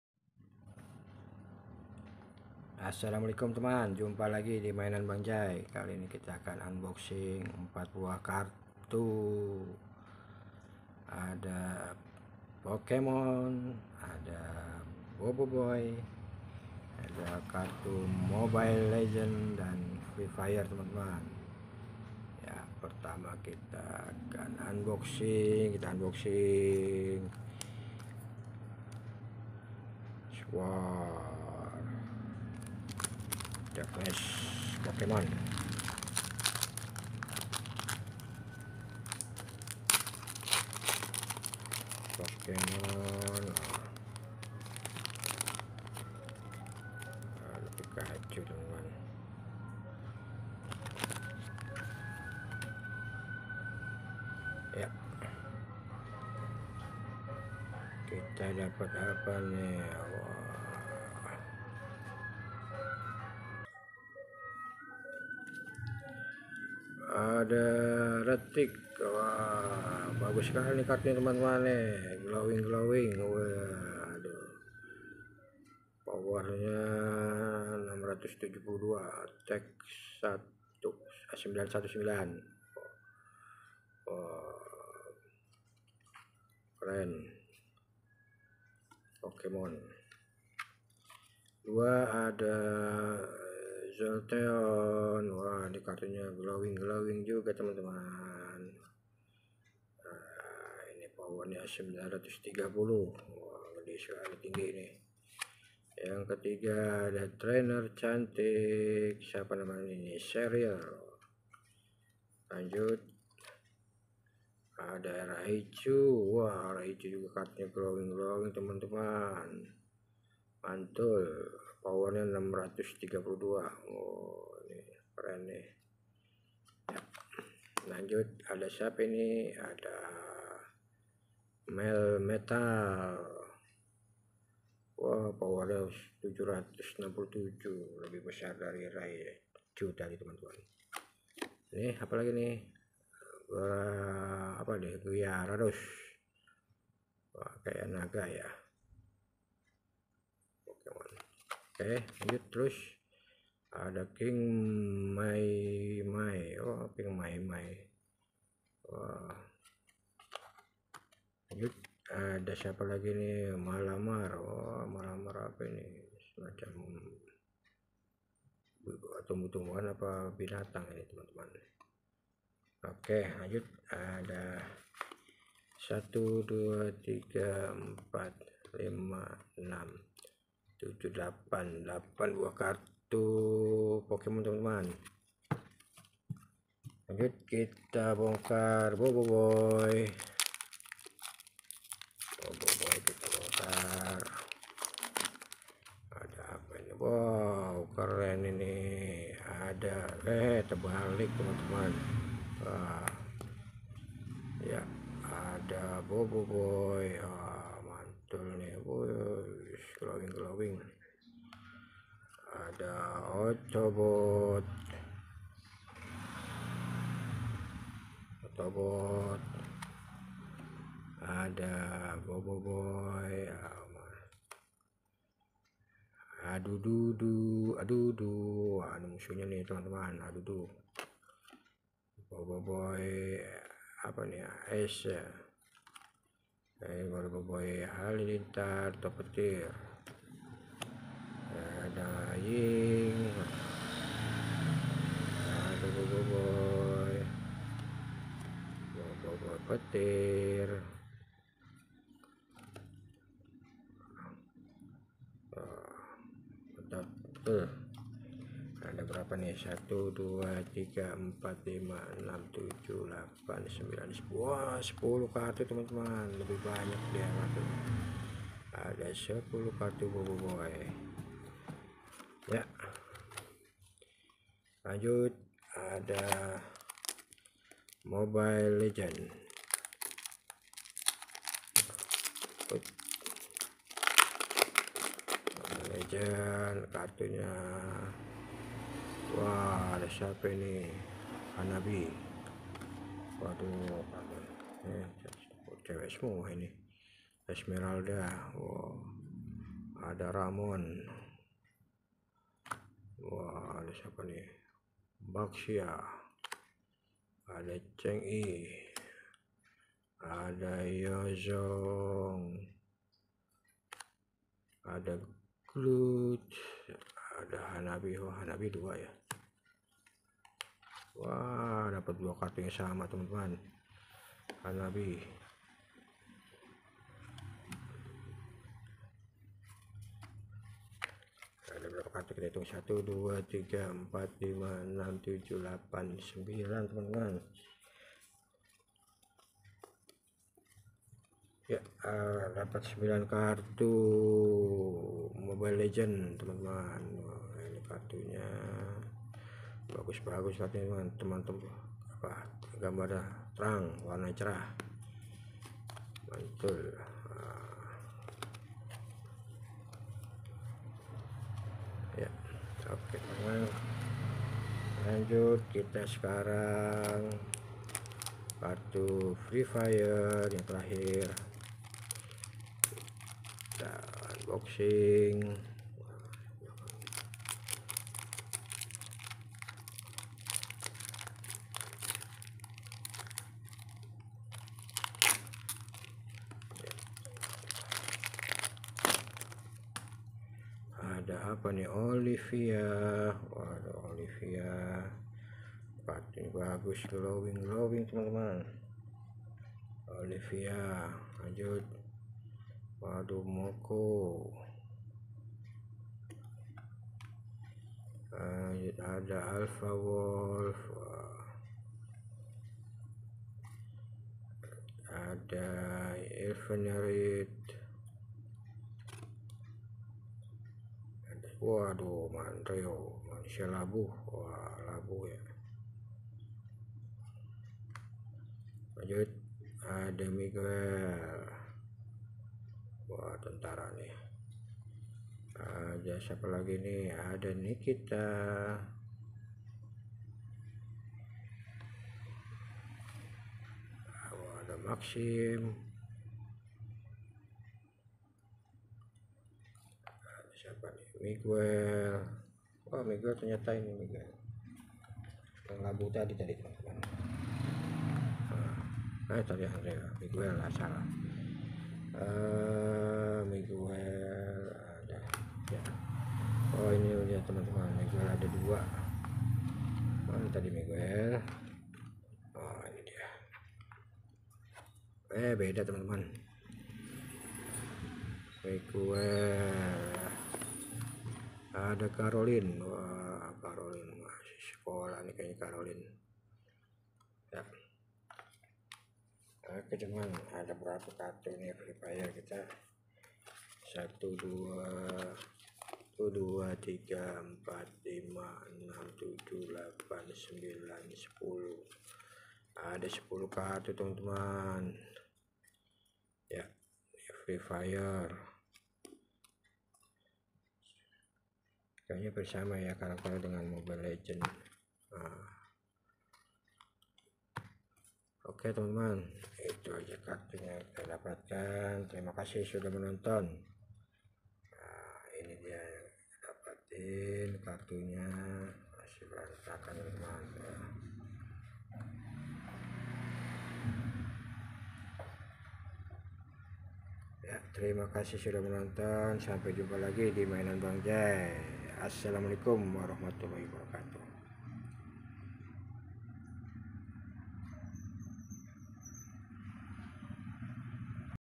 unboxing 4buah kartu lucu#mainankartu#koleksi@mainanbangja
unboxing 4buah kartu lucu#mainankartu#koleksi@mainanbangjay

unboxing empat buah kartu yaitu pokemon, mobile legends, freefire dan kartu boboiboy,
kartunya bagus bagus teman.